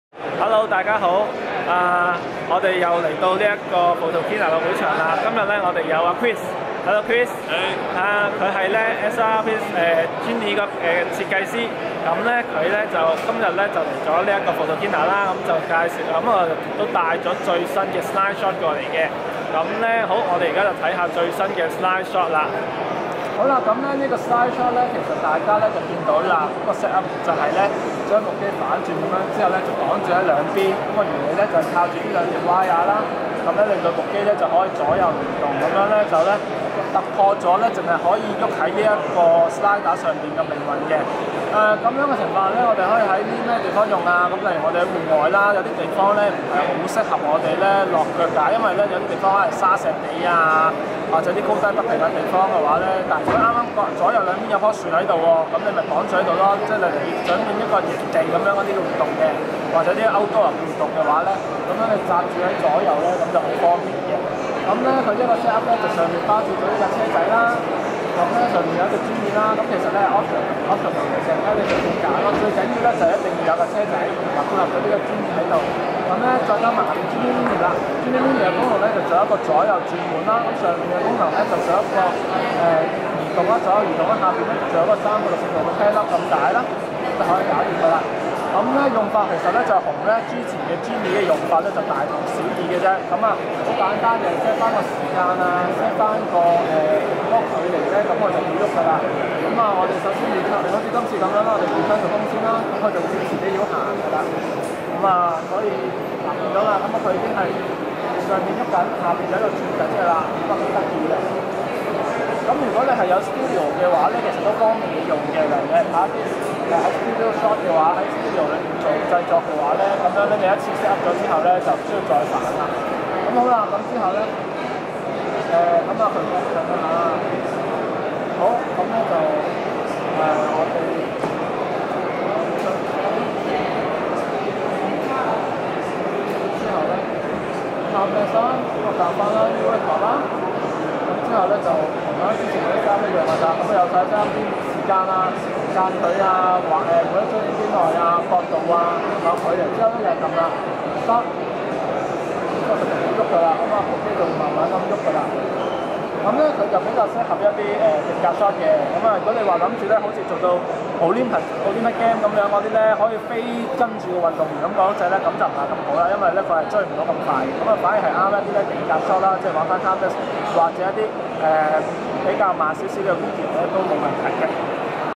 一首試 Syrp Sling Shot
Syrp 今次要飛上天啦！有了新的Sling Shot，就不用再DIY，不用再想帶那枝路軌出去了！可惜唔可以當Liner Cam 快拍影運動、動作場面，否則就真的不得了了！